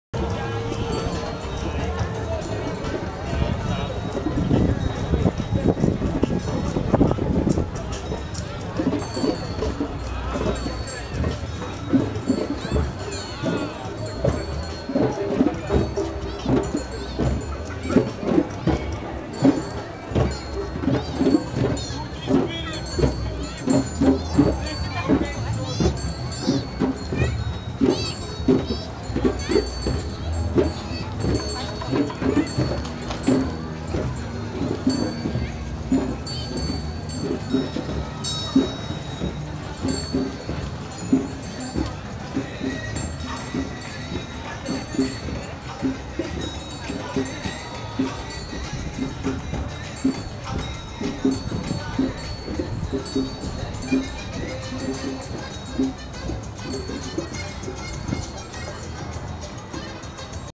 Kamelritt
Einmal Kamelreiten bei den Afrika-Tagen 2008 in Wien auf der Donauinsel.